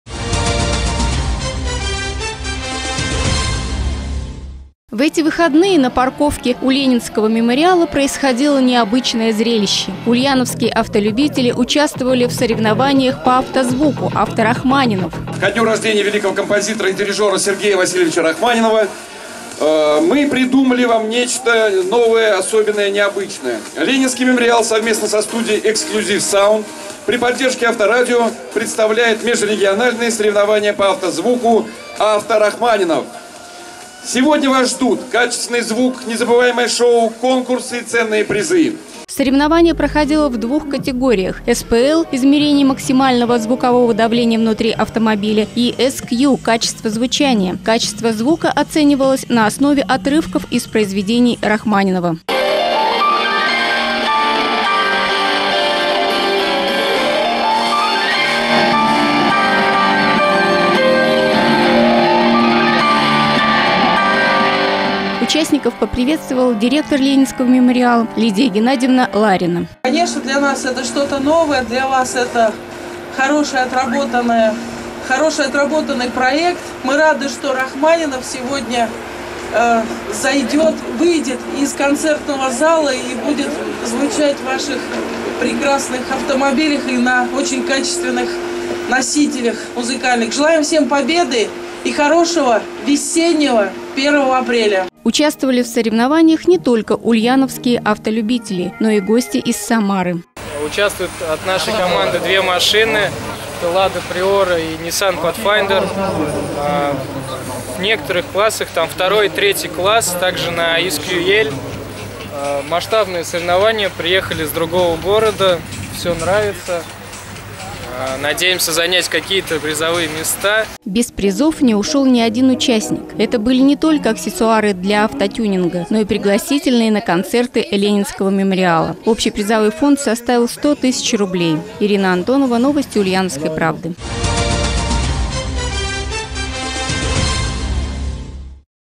В Ульяновске в день рождения Сергея Рахманинова устроили фестиваль автозвука
56 Международный музыкальный фестиваль «Мир, Эпоха, Имена...» - это не только музыка в залах! Ко дню рождения великого композитора и дирижера Сергея Васильевича Рахманинова Ленинский мемориал совместно со своими партнёрами организовал межрегиональные соревнования по автозвуку «АвтоРахманинов».